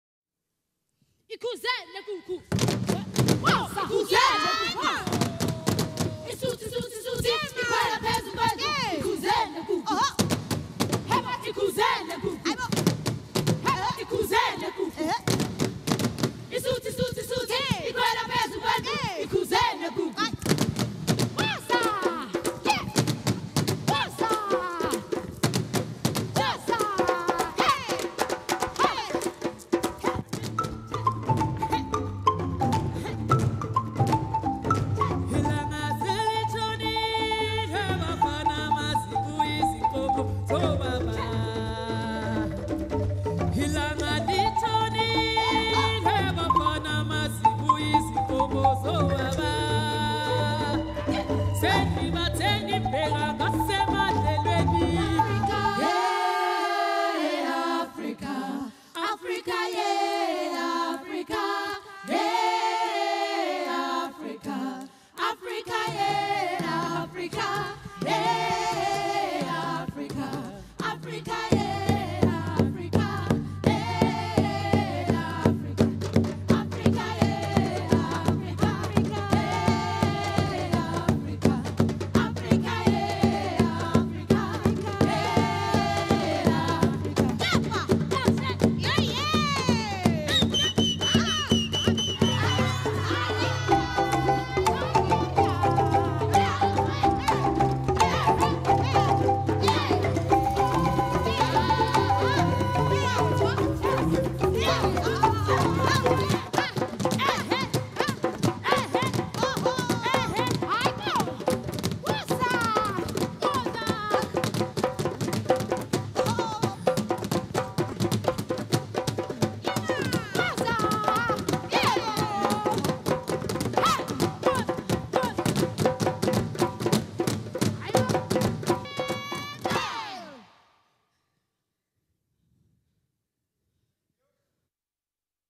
Abafazi African Sunset | do 16 Februari 2017 | Theater aan de Schie
In 2014 veroverde Abafazi hartverwarmend en zeer succesvol de Nederlandse theaters. Nu komen ze terug met een nieuwe show.  De negen Zuid-Afrikaanse vrouwen van Abafazi nemen het publiek mee op reis door de muziek en cultuur van Zuid-Afrika. De Afrikaanse drum, de marimba en hun stem zijn de basisinstrumenten.

Internationale hits van Miriam Makeba en Brenda Fassie passeren de revue, afgewisseld met indrukwekkende drum- en danspartijen en natuurlijk wordt er ook a capella gezongen. Een Afrika-belevenis om nooit te vergeten!